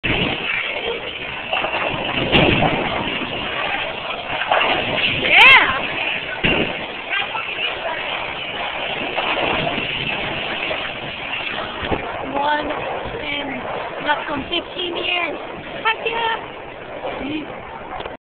Mom Bowling
my mom bowling after 15 years!!